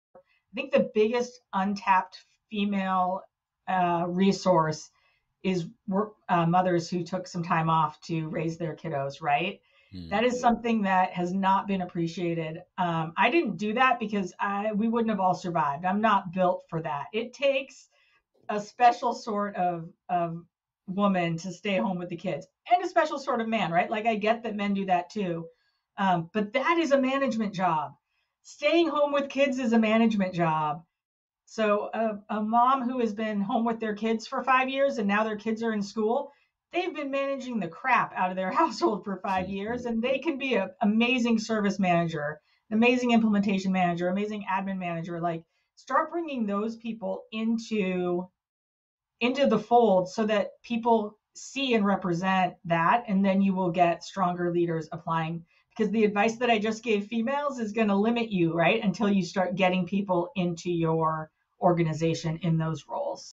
Biggest Untapped Female Resource
It is critical to have diversity in order to welcome new ideas and strategies as a company grows. There are currently few companies that promote female employees to higher positions in the corporate sector, and there are many untapped female resource categories that are still undervalued by some companies.

Join Lori as she takes you through her journey of becoming the CEO of PayNW.